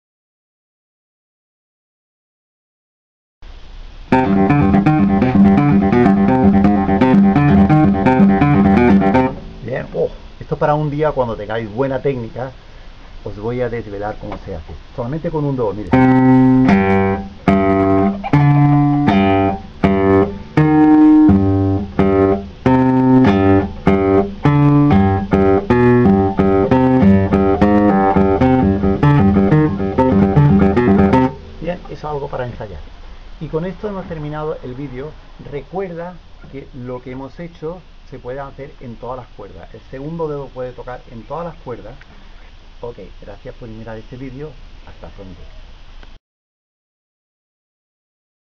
Velocidad extrema en el bajo eléctrico
Aprende tocar el bajo eléctrico con una velocidad extrema en poco tiempo (si no lo sabes ya)

Es un reto para el bajista, porque no trae casi nada de TAB y nada de jam track.

El autor posee todos los derechos del vídeo.